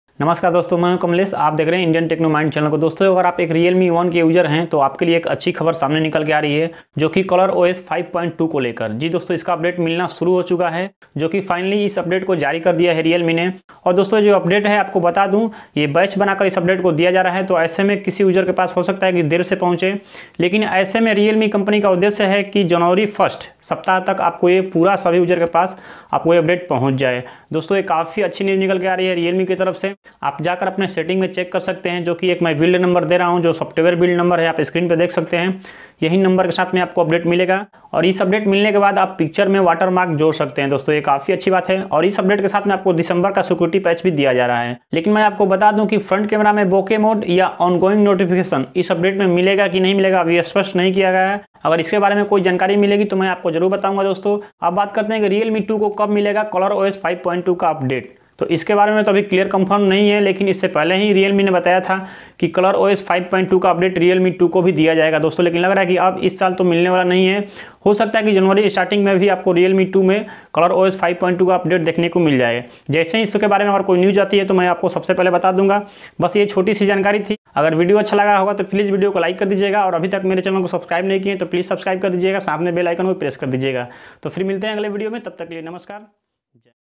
Realme 1 Official Software Update। Color Os 5.2,Realme 2 me kab milega Color Os 5.2 Update?
Numskar dosto pyar banaye rakhiyega

Dosto hum ish video me Realme 1 ke software update ke baare me baat kiye h. ummid h video pasand aayegi...